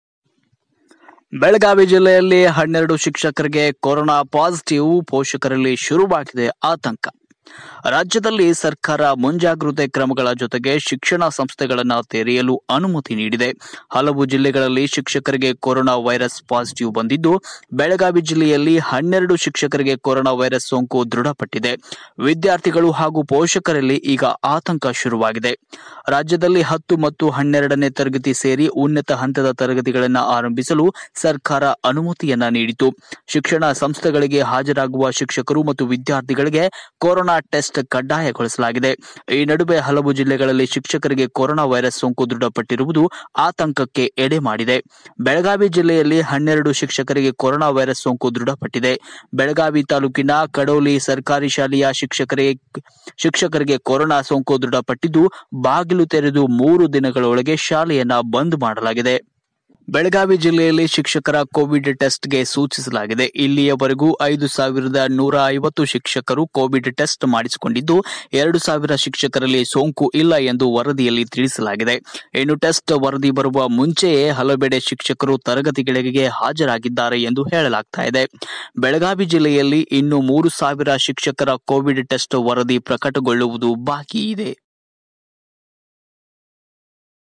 ಬೆಳಗಾವಿ ಜಿಲ್ಲೆಯ 12 ಶಿಕ್ಷಕರಿಗೆ ಕೋವಿಡ್ ದೃಢ: ಪಾಲಕರು, ಪೋಷಕರಲ್ಲಿ ಶುರುವಾಯ್ತು ಆತಂಕ..
Mr News Kannada 24×7

ರಾಜ್ಯದಲ್ಲಿ ಸರ್ಕಾರ ಮುಂಜಾಗ್ರತೆ ಕ್ರಮಗಳ ಜೊತೆಗೆ ಶಿಕ್ಷಣ ಸಂಸ್ಥೆಗಳನ್ನು ತೆರೆಯಲು ಅನುಮತಿ ನೀಡಿದೆ. ಹಲವು ಜಿಲ್ಲೆಗಳಲ್ಲಿ ಶಿಕ್ಷಕರಿಗೆ ಕೊರೊನಾ ವೈರಸ್ ಪಾಸಿಟಿವ್ ಬಂದಿದೆ. ಬೆಳಗಾವಿ ಜಿಲ್ಲೆಯಲ್ಲಿ 12 ಶಿಕ್ಷಕರಿಗೆ ಕೊರೊನಾ ವೈರಸ್ ಸೋಂಕು ದೃಢಪಟ್ಟಿದೆ. ವಿದ್ಯಾರ್ಥಿಗಳು, ಪೆÇೀಷಕರಲ್ಲಿ ಆತಂಕ ಶುರುವಾಗಿದೆ.

ರಾಜ್ಯದಲ್ಲಿ 10 ಮತ್ತು 12ನೇ ತರಗತಿ ಸೇರಿ ಉನ್ನತ ಹಂತದ ತರಗತಿಗಳನ್ನು ಆರಂಭಿಸಲು ಸರ್ಕಾರ ಅನುಮತಿ ನೀಡಿದೆ. ಶಿಕ್ಷಣ ಸಂಸ್ಥೆಗಳಿಗೆ ಹಾಜರಾಗುವ ಶಿಕ್ಷಕರು ಮತ್ತು ವಿದ್ಯಾರ್ಥಿಗಳಿಗೆ ಕೋವಿಡ್ ಟೆಸ್ಟ್ ಕಡ್ಡಾಯಗೊಳಿಸಲಾಗಿದೆ. ಈ ನಡುವೆ ಹಲವು ಜಿಲ್ಲೆಗಳಲ್ಲಿ ಶಿಕ್ಷಕರಿಗೆ ಕೊರೊನಾ ವೈರಸ್ ಸೋಂಕು ದೃಢಪಟ್ಟಿರುವುದು ಆತಂಕಕ್ಕೆ ಎಡೆಮಾಡಿದೆ. ಬೆಳಗಾವಿ ಜಿಲ್ಲೆಯಲ್ಲಿ 12 ಶಿಕ್ಷಕರಿಗೆ ಕೊರೊನಾ ವೈರಸ್ ಸೋಂಕು ದೃಢಪಟ್ಟಿದೆ. ಬೆಳಗಾವಿ ತಾಲೂಕಿನ ಕಡೋಲಿ ಸರ್ಕಾರಿ ಶಾಲೆಯ ಶಿಕ್ಷಕರಿಗೆ ಕೊರೊನಾ ಸೋಂಕು ದೃಢಪಟ್ಟಿದ್ದು, ಬಾಗಿಲು ತೆರೆದು ಮೂರು ದಿನಗಳೊಳಗೆ ಶಾಲೆಯನ್ನು ಬಂದ್ ಮಾಡಲಾಗಿದೆ.

ಬೆಳಗಾವಿ ಜಿಲ್ಲೆಯಲ್ಲಿ ಶಿಕ್ಷಕರ ಕೋವಿಡ್ ಟೆಸ್ಟ್‍ಗೆ ಸೂಚಿಸಲಾಗಿದೆ. ಇಲ್ಲಿಯವರೆಗೆ 5150 ಶಿಕ್ಷಕರ ಕೋವಿಡ್ ಟೆಸ್ಟ್ ಆಗಿದ್ದು, 2 ಸಾವಿರ ಶಿಕ್ಷಕರಲ್ಲಿ ಸೋಂಕು ಇಲ್ಲ ಎಂದು ವರದಿಯಲ್ಲಿ ತಿಳಿಸಲಾಗಿದೆ. ಇನ್ನು ಟೆಸ್ಟ್ ವರದಿ ಬರುವ ಮುಂಚೆಯೇ ಹಲವೆಡೆ ಶಿಕ್ಷಕರು ತರಗತಿಗಳಿಗೆ ಹಾಜರಾಗಿದ್ದಾರೆ ಎಂದು ಹೇಳಲಾಗುತ್ತಿದೆ. ಬೆಳಗಾವಿ ಜಿಲ್ಲೆಯಲ್ಲಿ ಇನ್ನೂ 3 ಸಾವಿರ ಶಿಕ್ಷಕರ ಕೋವಿಡ್ ಟೆಸ್ಟ್ ವರದಿ ಪ್ರಕಟಗೊಳ್ಳುವುದು ಬಾಕಿ ಇದೆ.